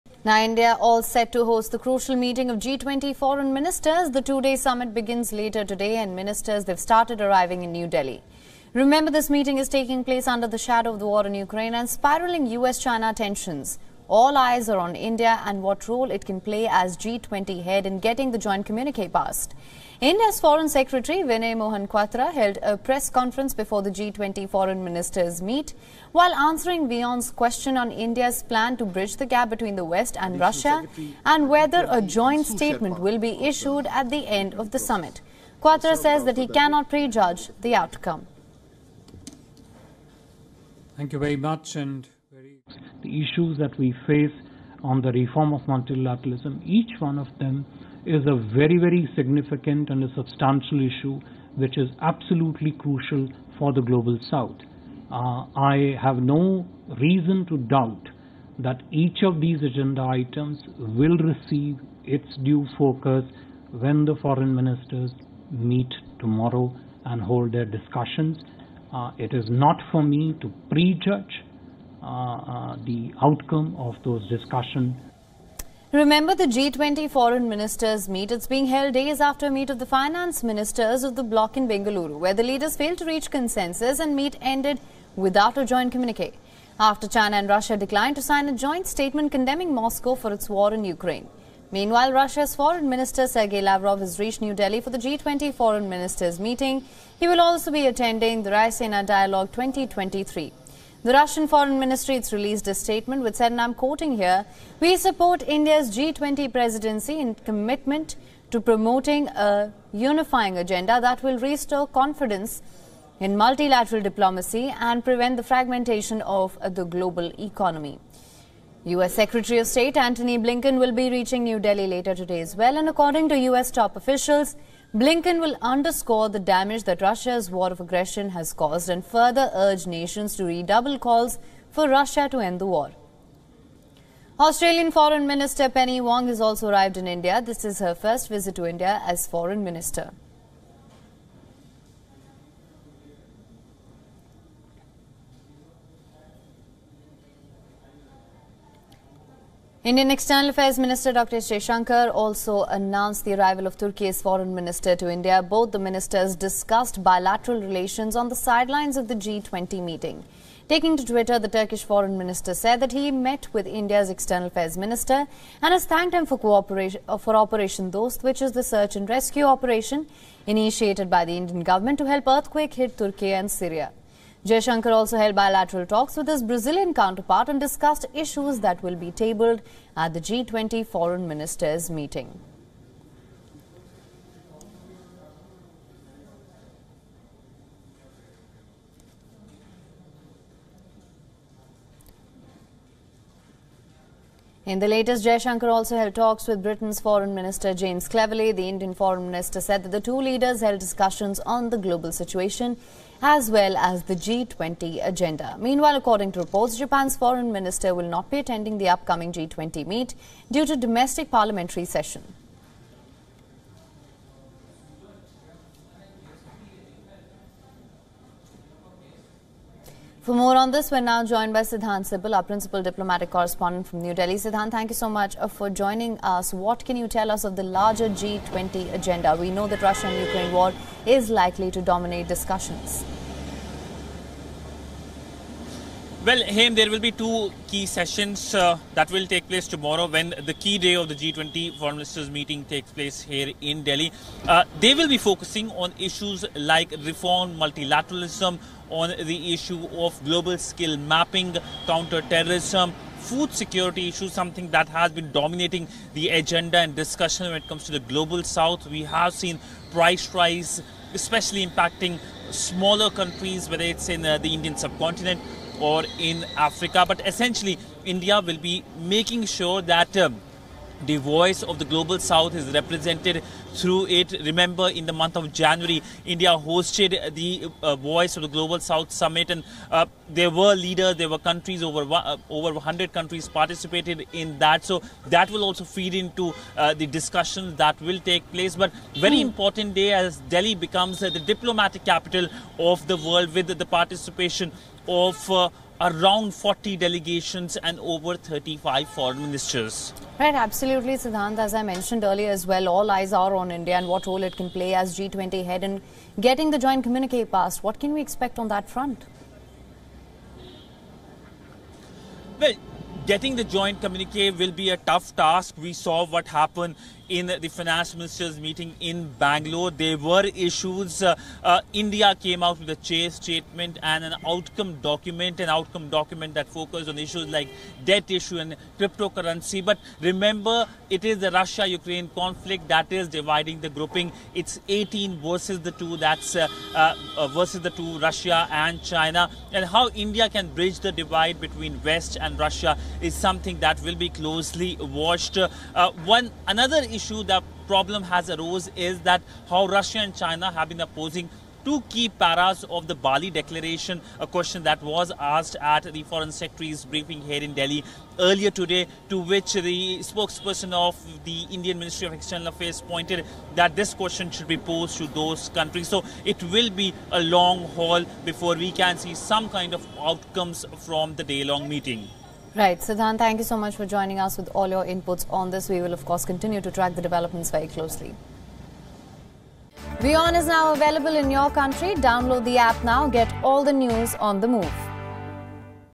Indian Foreign Secretary holds press conference ahead of G20 meet I Latest News I WION
India's foreign secretary Vinay Mohan Kwatra held a press conference before the G20 Foreign ministers meet. Foreign ministers from around the world will attend the G20 Summit which is taking place in New Delhi.